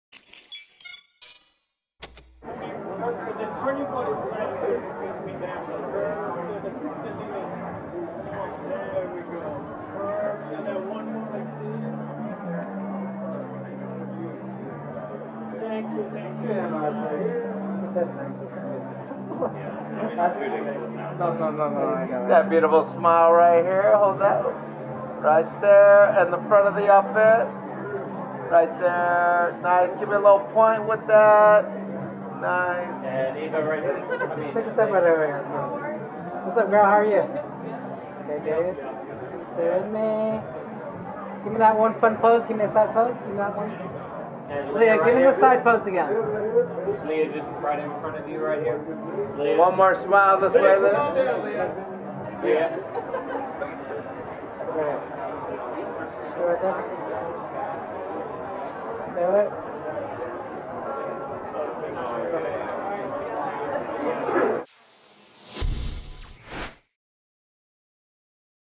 Leah Talabi attends 'Darren Dzienciol’s Haunted Hotel' red carpet
B-roll footage: Leah Talabi on the red carpet at Darren Dzienciol’s Haunted Hotel in partnership with Utopia and presented by Casa Azul Tequila at The Fairmont Century City on Friday, November 1st, 2024.